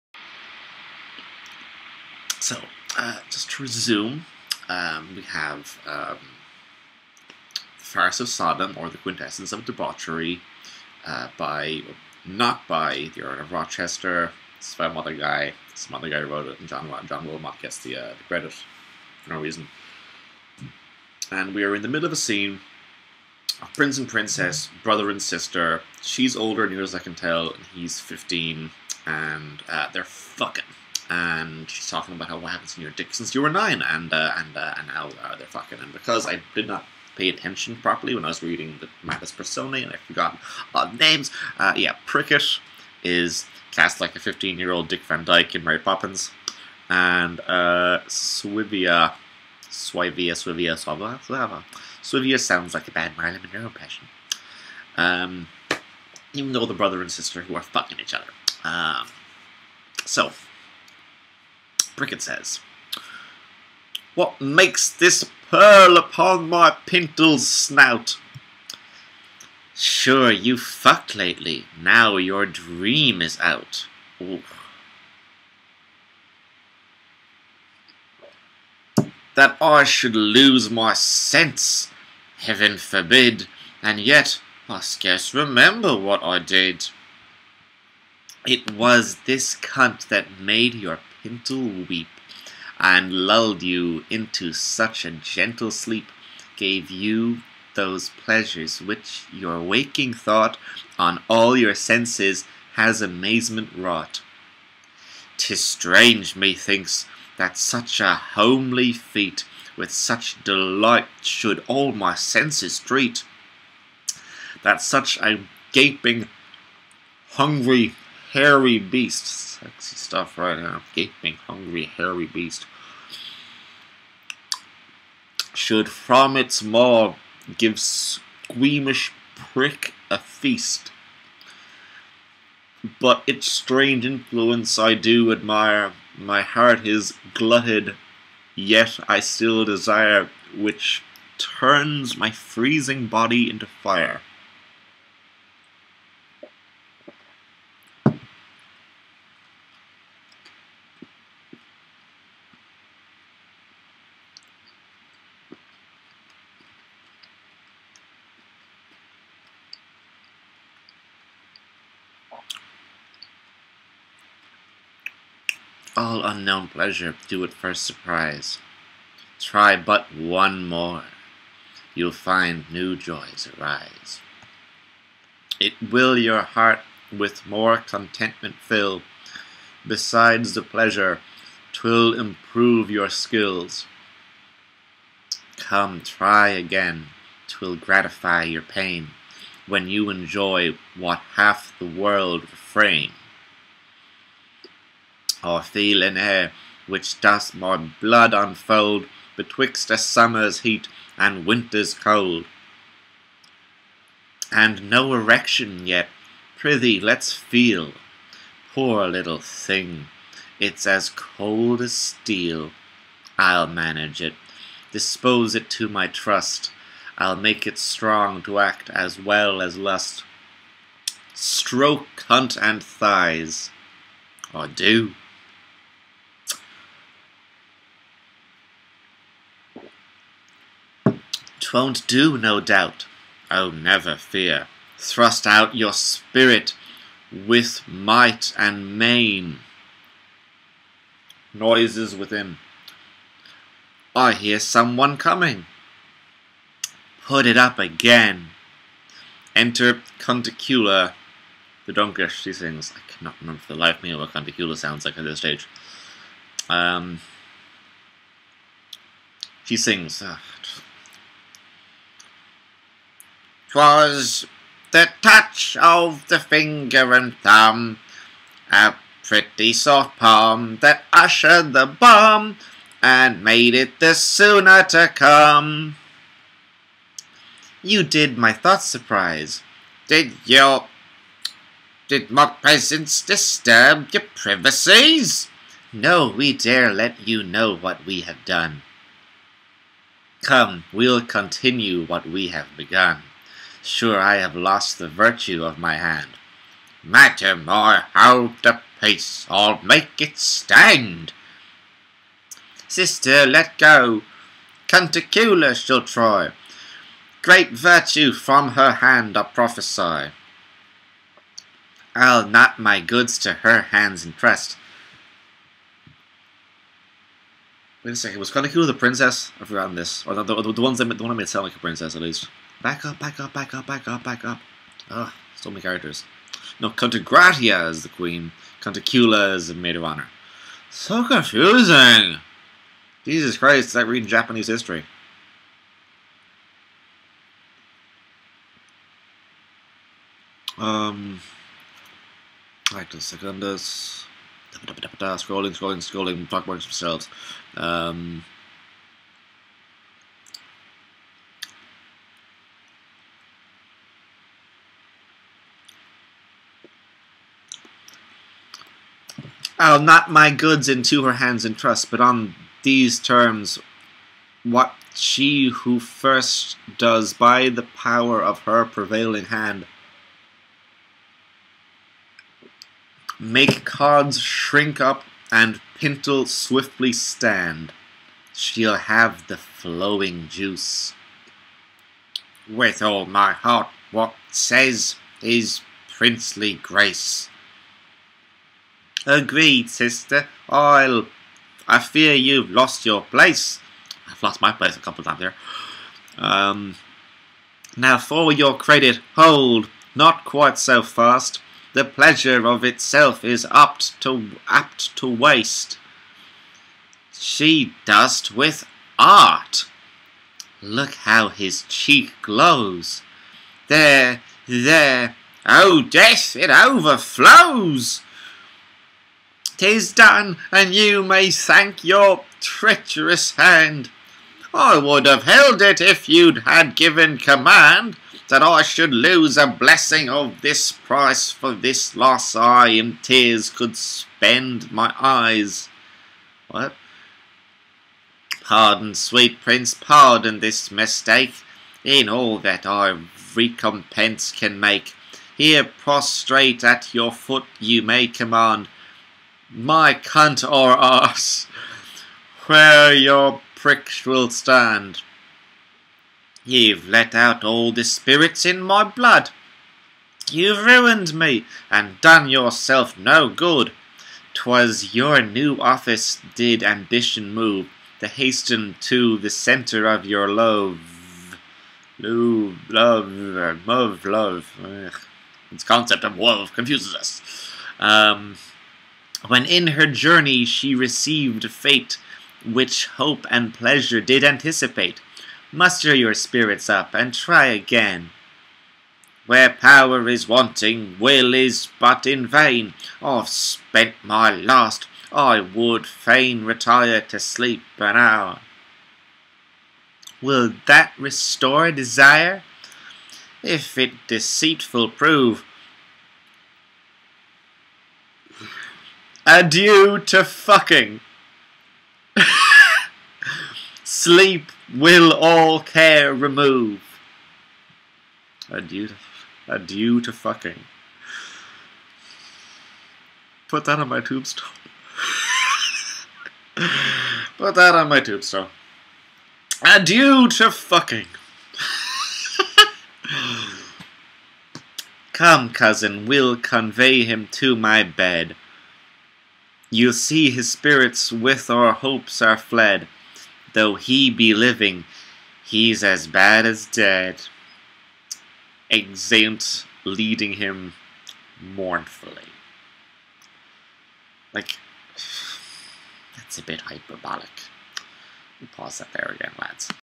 The Quintessence of Debauchery, cold reading, part 5
The Farce of Sodom, or The Quintessence of Debauchery, not by John Wilmot.

I make no apologies for my terrible elocution and penchant for eddying of attention.